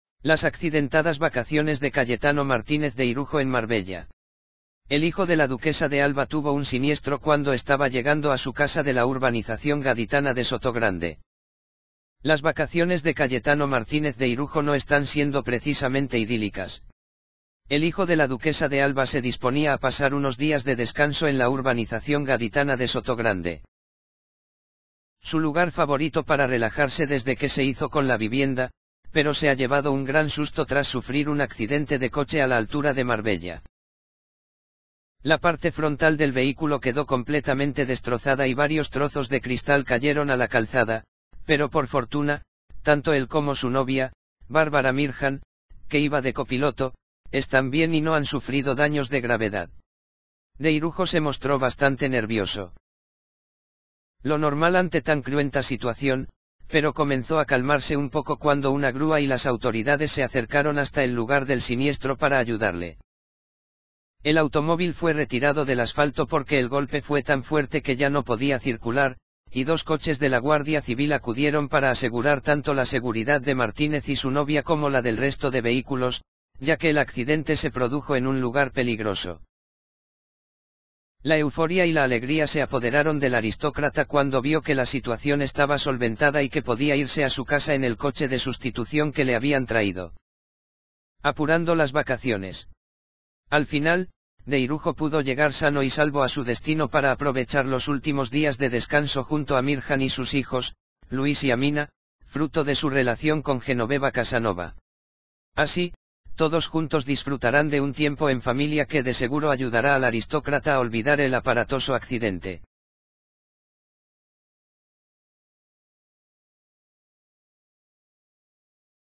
Las accidentadas vacaciones de Cayetano Martínez de Irujo en Marbella
El hijo de la Duquesa de Alba tuvo un siniestro cuando estaba llegando a su casa de la urbanización gaditana de Sotogrande.